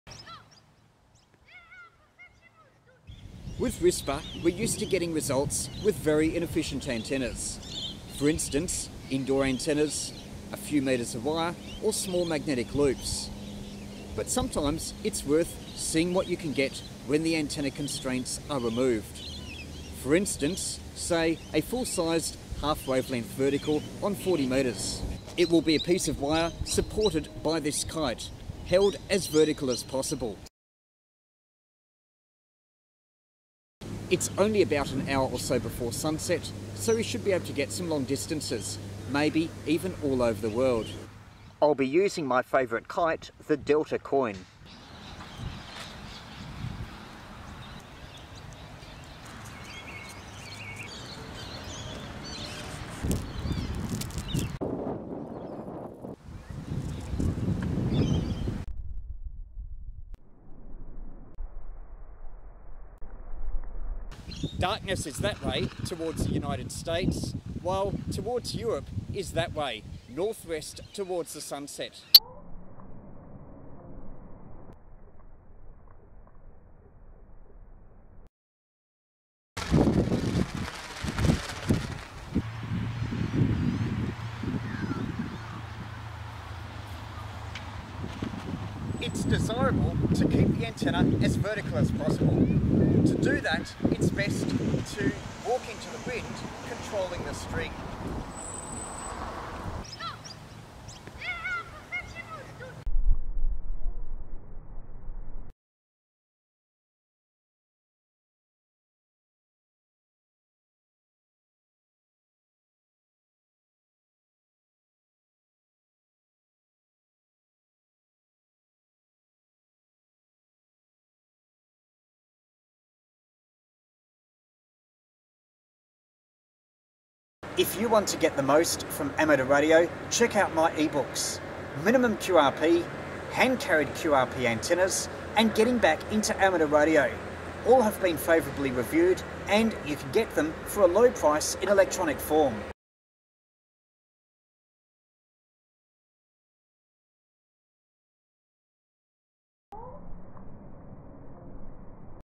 7 MHz WSPR with a kite supported vertical antenna
WSPR can work with inefficient antennas. But what if you use a good antenna - such as a half wavelength vertical? Video shows a test with a kite supported wire antenna from a local park.  Exploiting greyline propagation, 200mW on 7 MHz is sufficient to be spotted as far away as North America and the Canary Islands.